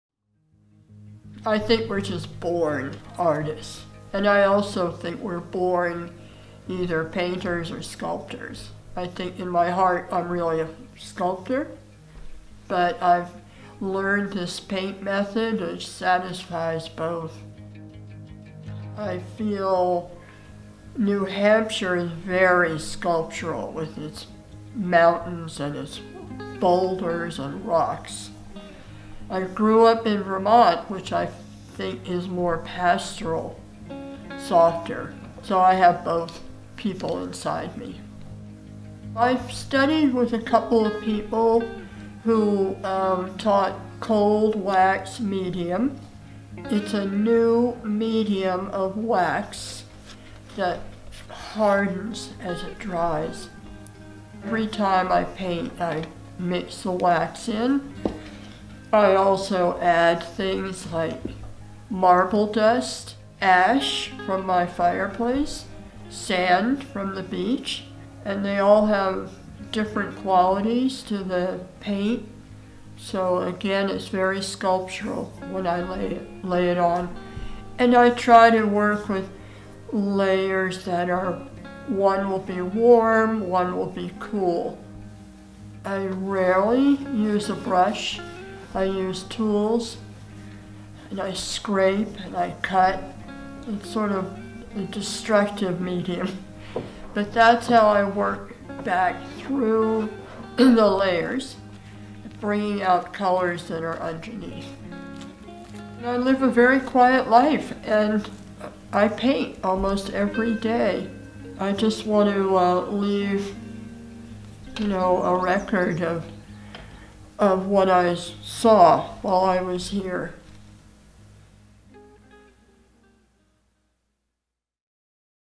Ewing Arts Awards 2022: Jessie Pollock
Painter Jessie Pollock is the recipient of a 2022 Ewing Arts Award in the two-dimensional arts category. Video by Hannah Schroeder / Sentinel Staff.